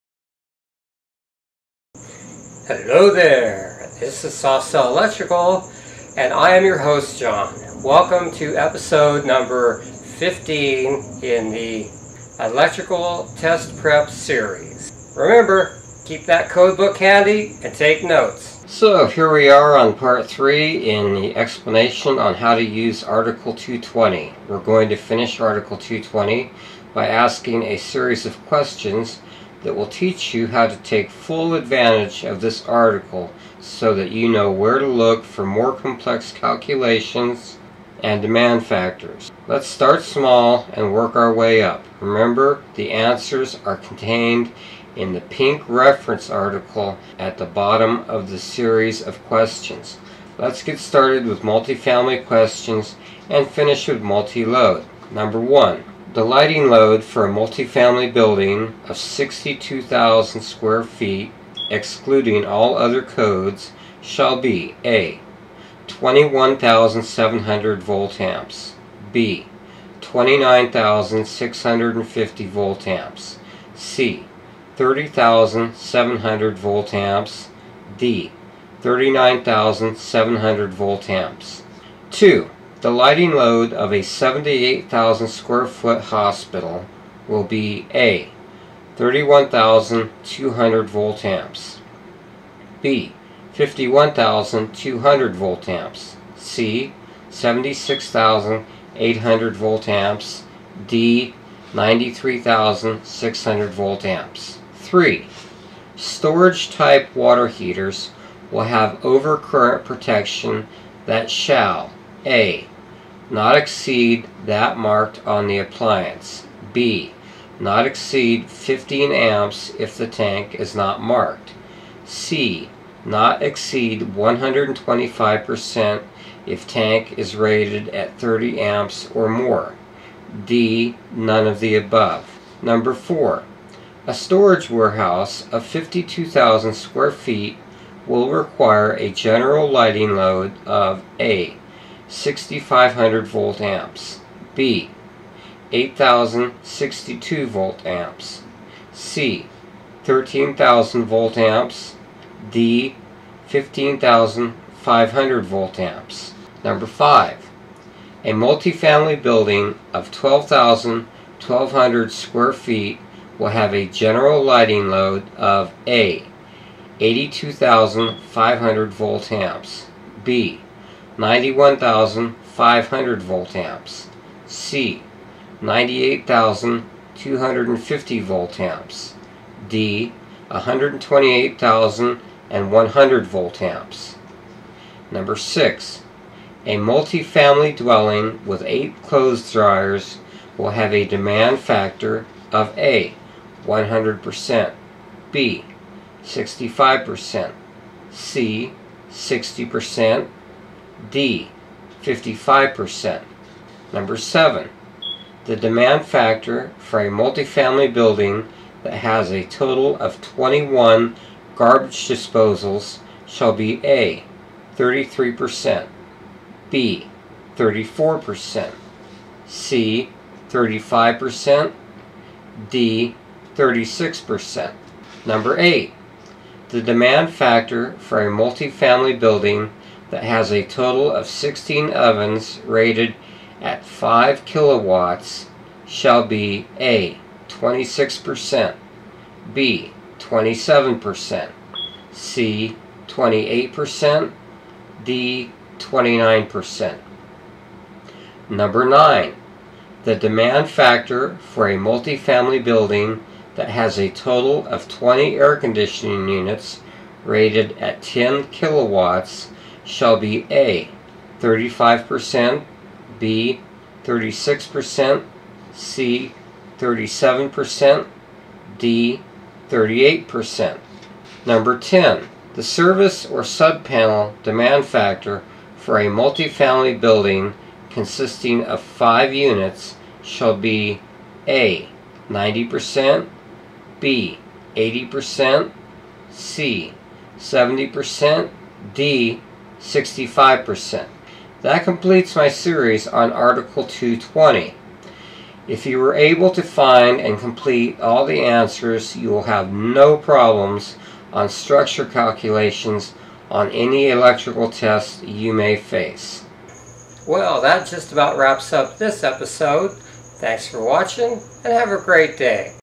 NEC code # 7 article 220, # 15
This covers article 220, which shows you how to effectively use the NEC code book to make calculations on multifamily buildings as well as applying usage factors.